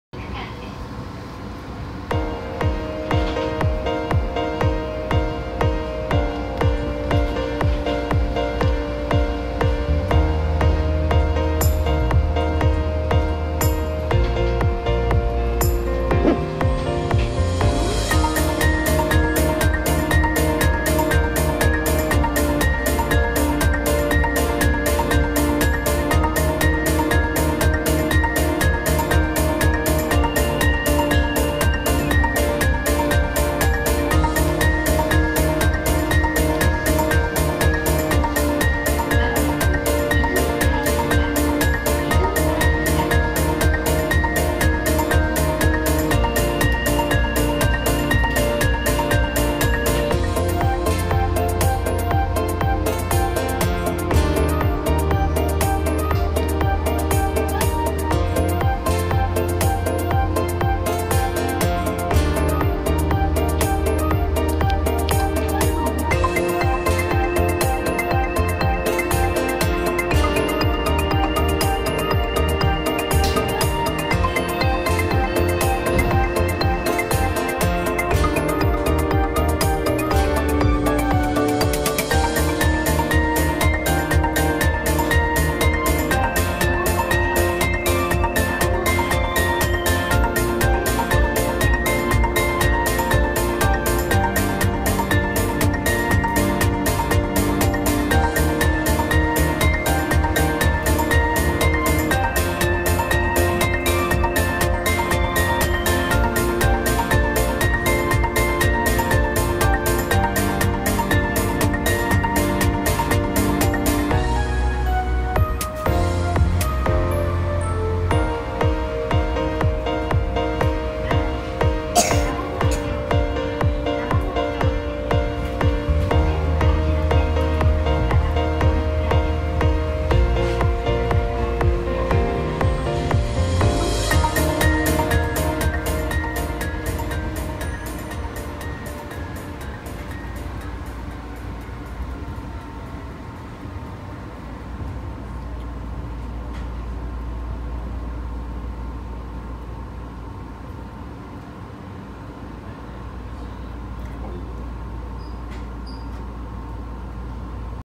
【鉄道の旅】Monorail レオ同乗 多摩モノレール 西武ライオンズ優勝おめでとう！ Lions レオくんと前面展望 立飛駅～玉川上水駅
西武ライオンズ優勝おめでとう！
多摩モノレールです。
良い天気です。
野球 Lionsのレオがいました！